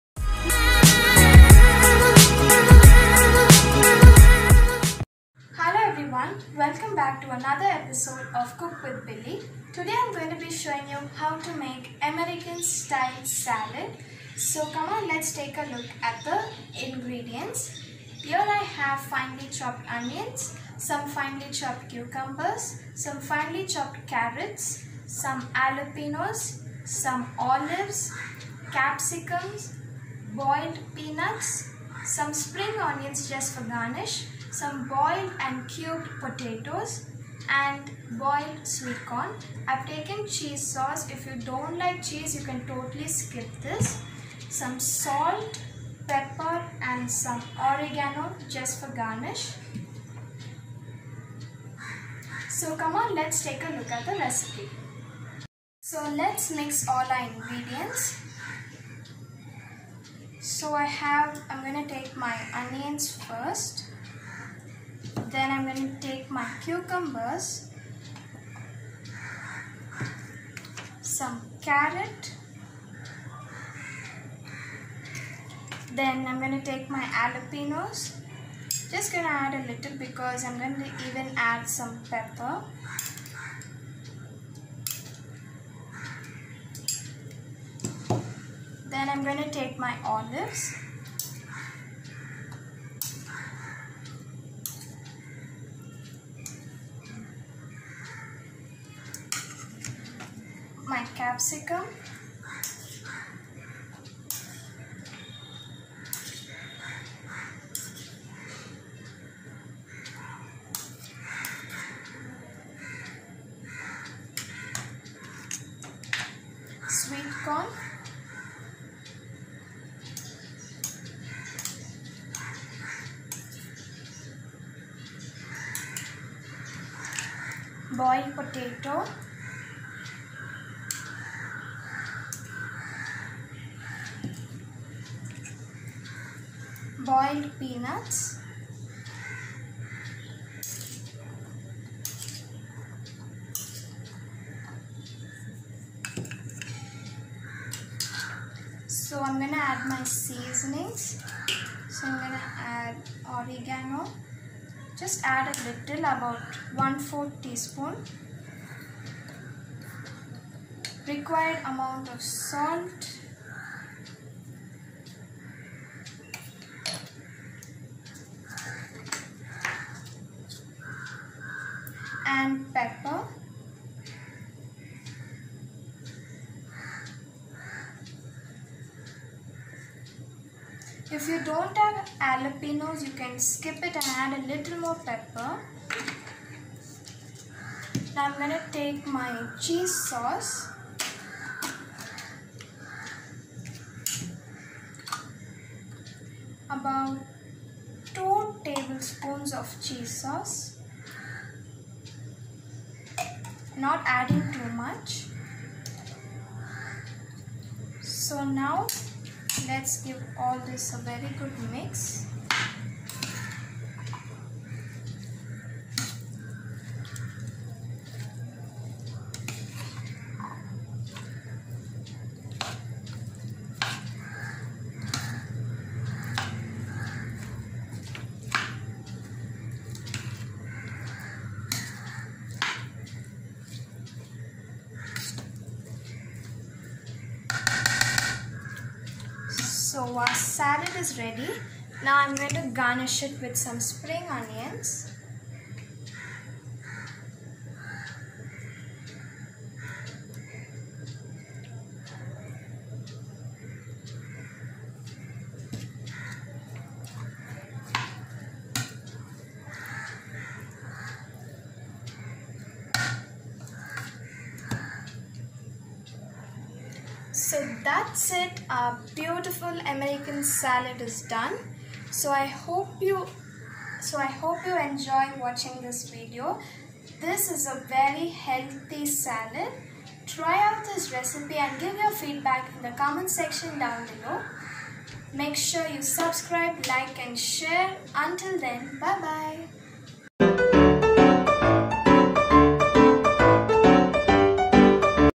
American style salad| Healthy and yummy salad|episode 3 of cook with billy
Hello guys I am back with a yummy recipe hope u try and give ur feedback in the comment section down below
INGREDIENTS
1. Onions
2. Carrot
3.Cucumber
4. Capsicum
5. Olives
6. Jalapenos
7. Boiled Peanuts
8. Boiled potato
9. Boiled corn
10. Salt
11. Pepper
12. Oregano
13. Cheese Sauce